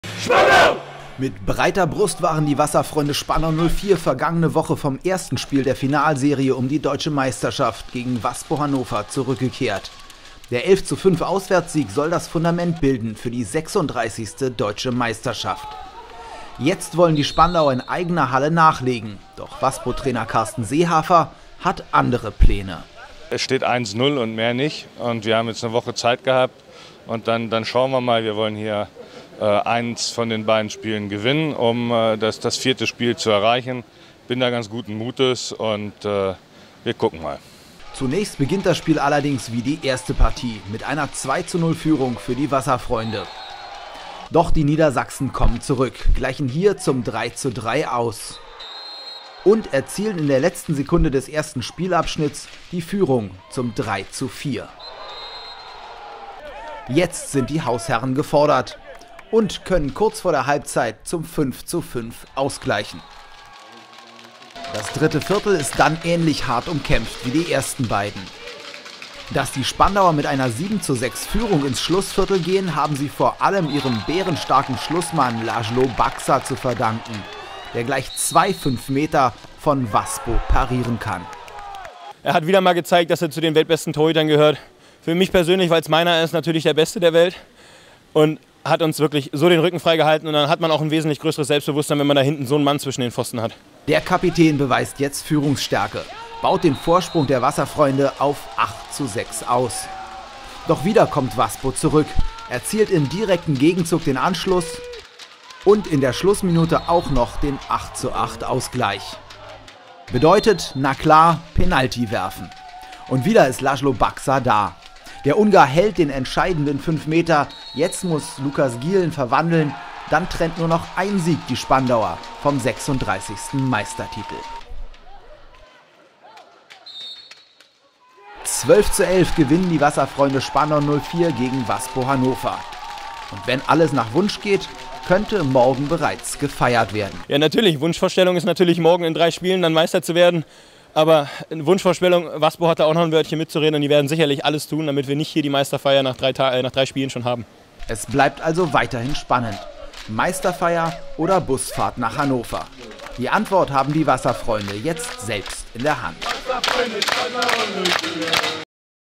DWL Play-Off Finale | Wasserfreunde Sp 04 vs Waspo Hannover WBTV
Mit breiter Brust waren die Wasserfreunde Spandau 04 vergangene Woche vom ersten Spiel der Finalserie um die Deutsche Meisterschaft gegen WASPO Hannover zurückgekehrt. Der 11:5 Auswärtssieg soll das Fundament bilden für die 36. Deutsche Meisterschaft seit 1979. Jetzt wollen die Spandauer in eigener Halle nachlegen, doch WASPO Trainer Karsten Seehafer hat andere Pläne.